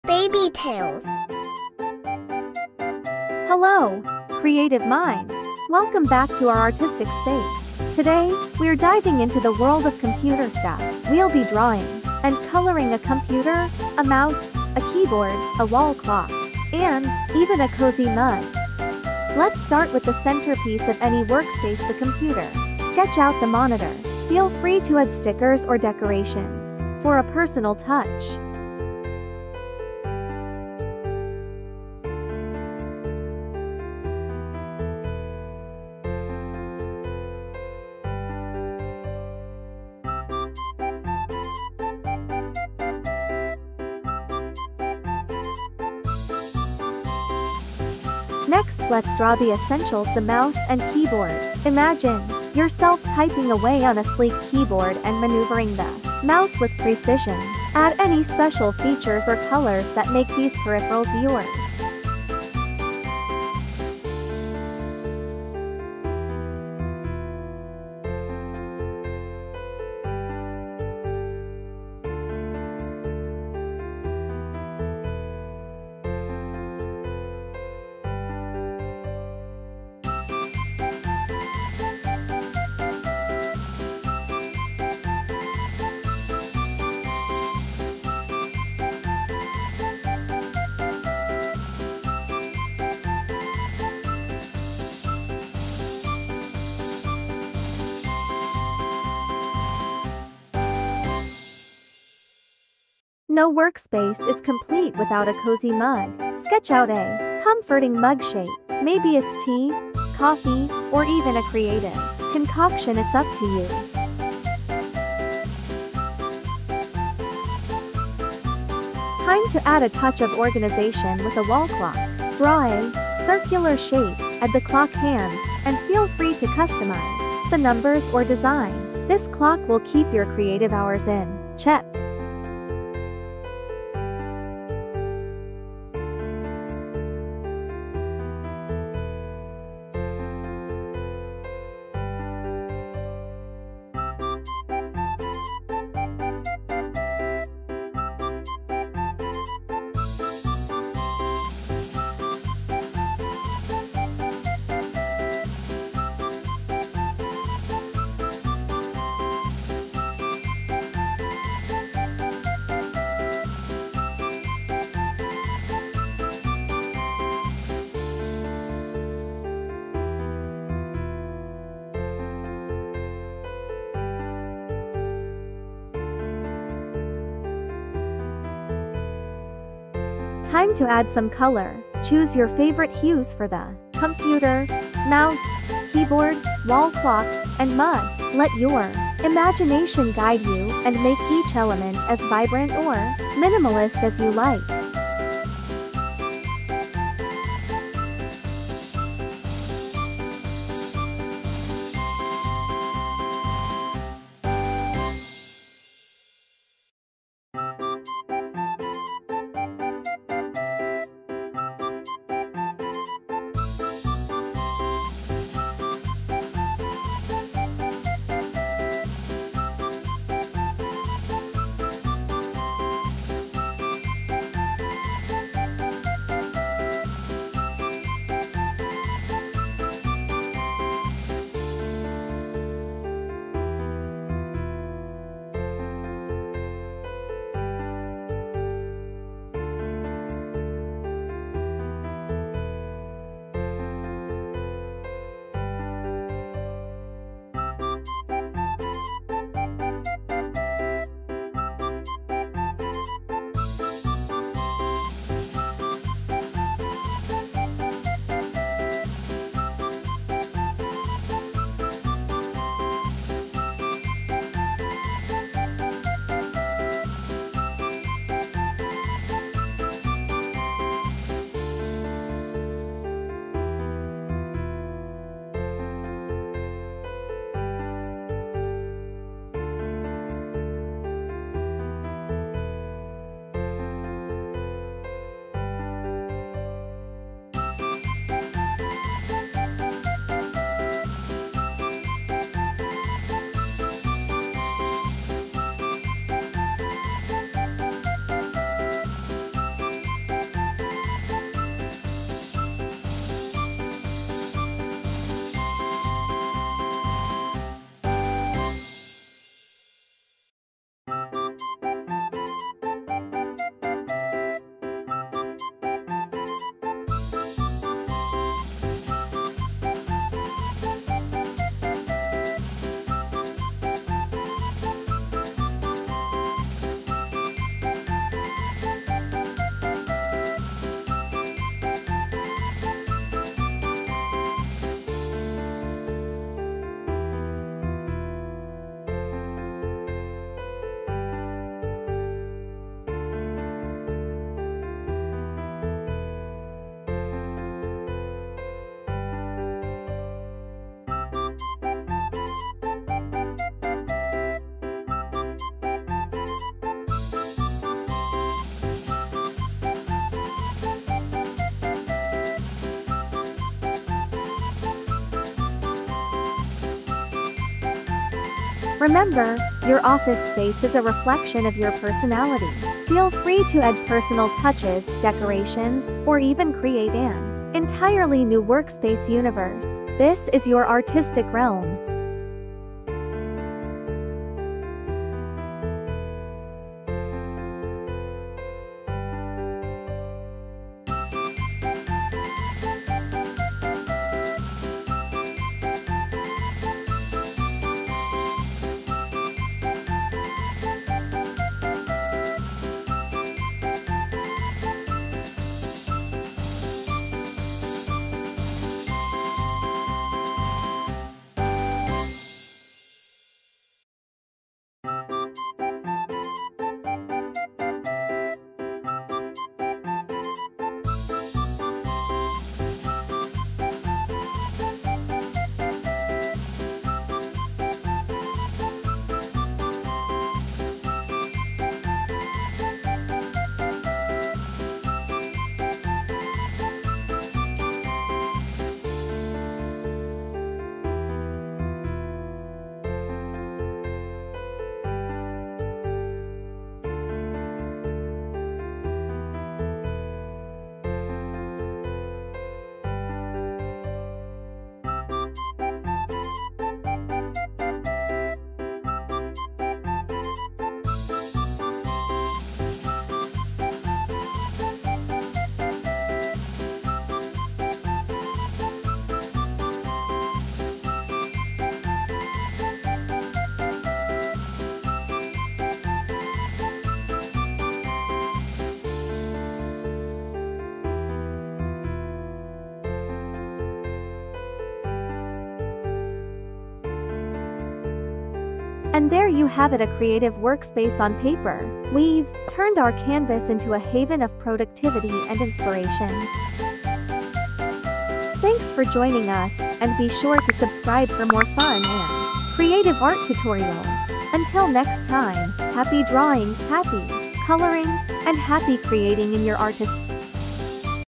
Computer, Mouse, Keyboard Drawing and Coloring for Kids and Toddlers | Art Fun
🎨✏️ Step into the world where art meets technology! Join us in this creative endeavor as we draw and color essential office items – a computer, a mouse, a keyboard, a wall clock, and a cozy mug. Transform your artistic space into a haven of productivity and style.

💻🖱️🕰️ Grab your art supplies – paper, colored pencils, markers, or crayons – and let's bring these office essentials to life. From sketching out the sleek lines of the computer to adding personalized details to the wall clock, every stroke is a step closer to creating your dream workspace.

☕️🌈 Whether you're a tech enthusiast or just looking for a fun and creative project, this tutorial is designed to inspire and encourage your artistic journey. Personalize your office elements, add decorations, or even create an entirely new workspace universe.

Let's turn our artistic canvas into a space where technology and creativity coexist! 🎨✏️💻🖱️